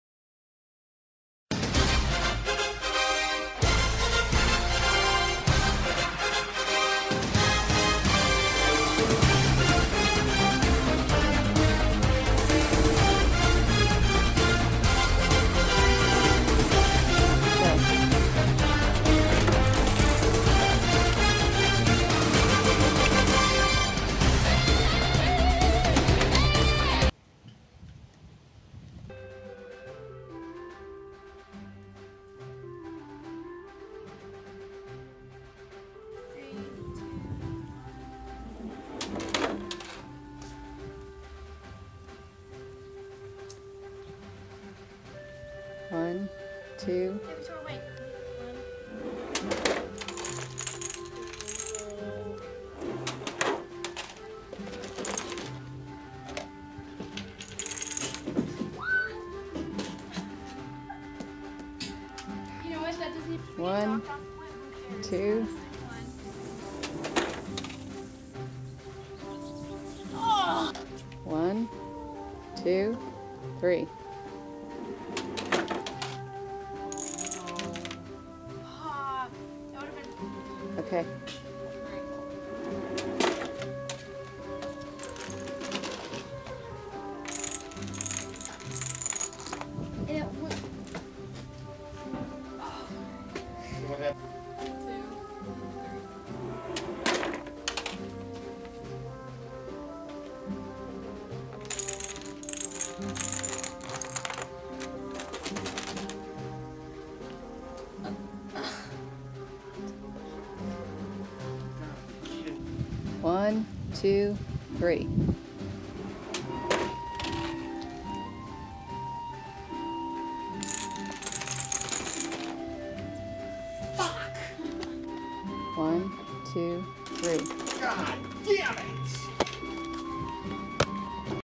Rube Goldberg Project- Vanessa and Victoria
A realist's point of view on the dreaded "Rube Goldberg Project". Rated PG-13 for Language.